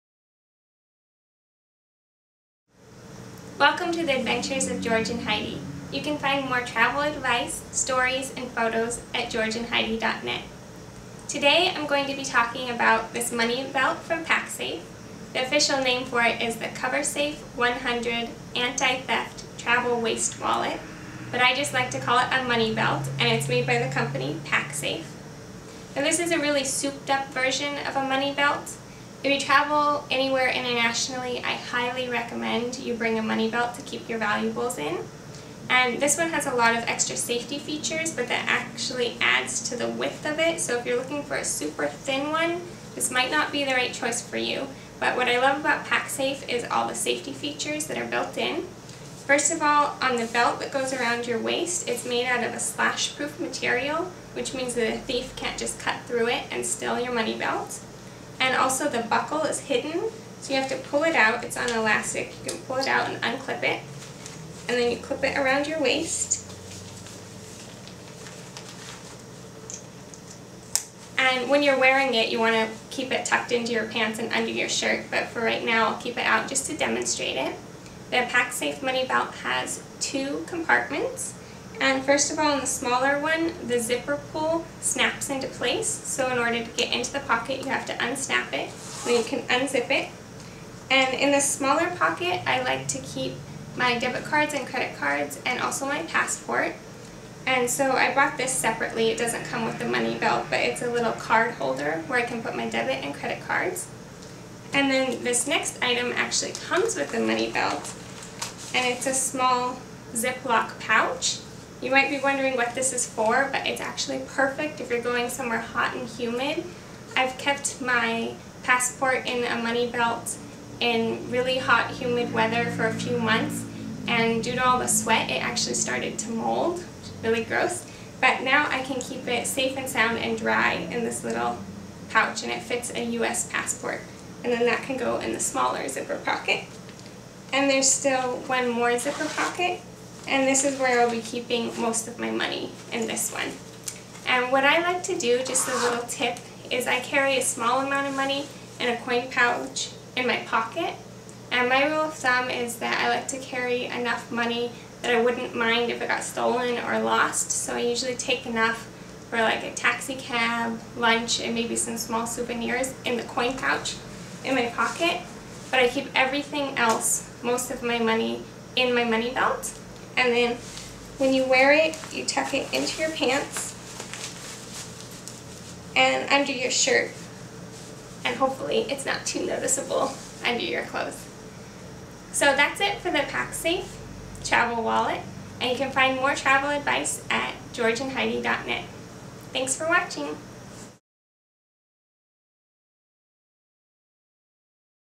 PacSafe Coversafe 100 Anti-Theft Travel Waist Wallet Review
Review of the PacSafe money belt for protecting valuables while traveling.

Follow the Adventures of George and Heidi